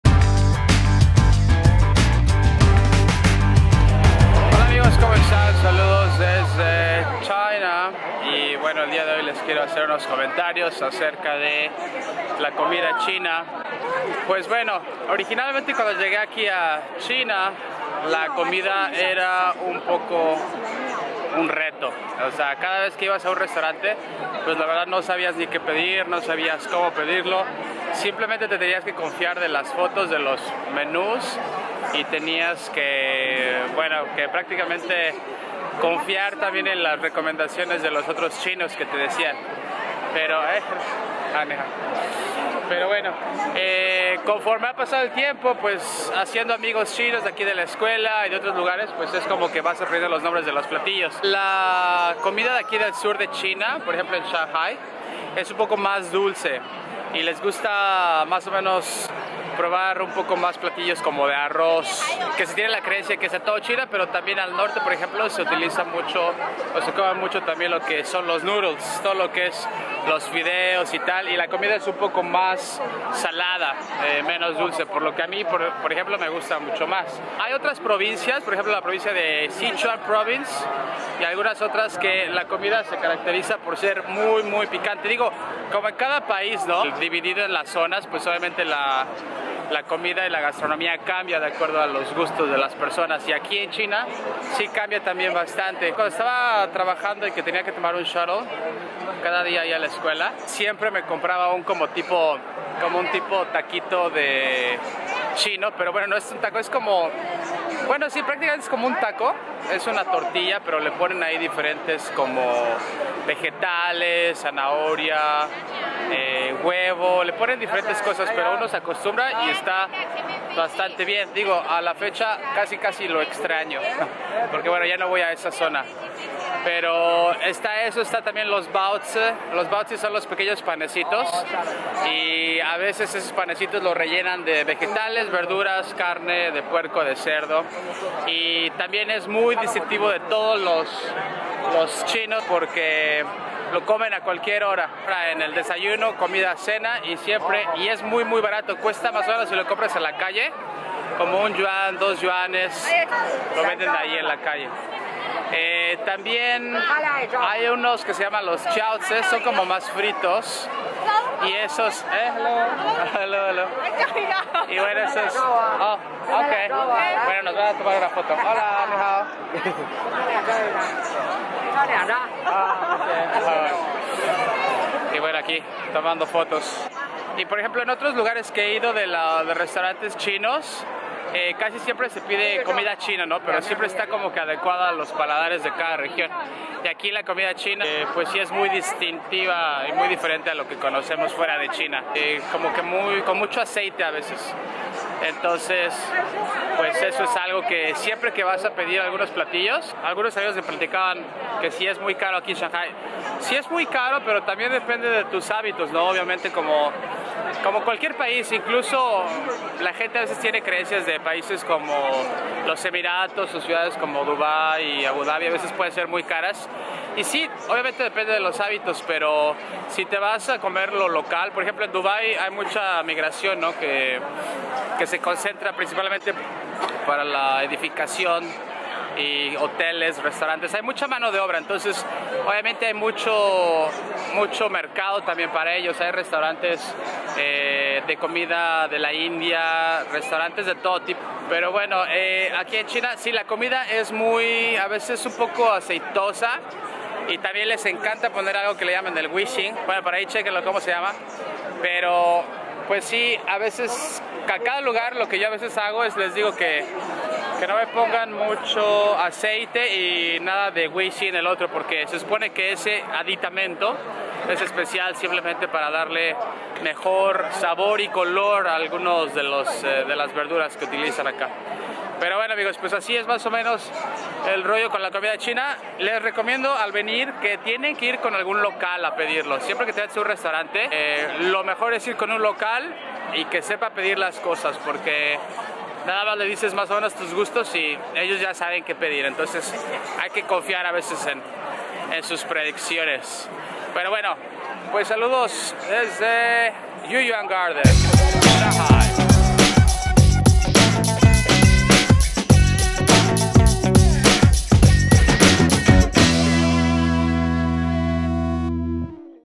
Cómo es la comida en China | Video Comida China en Shanghai
La comida en China es llamémosle interesante. Siendo China uno de los países más grandes del mundo, la variedad entre sus provincias es inmensa, así como cultura, gusto y comida.

La comida china en shanghai suele ser más dulce que en el norte en lugares como Beijing, etc.  También ha habido gran demanda en los últimos años como la comida rápida y grandes cadenas se han apropiados de lugares claves para la venta de comida rápida China.

Me encuentro en el yu yuan garden en Shanghai y les presento unos pequeños jiaozis y baozis de la zona. Muy recomendable!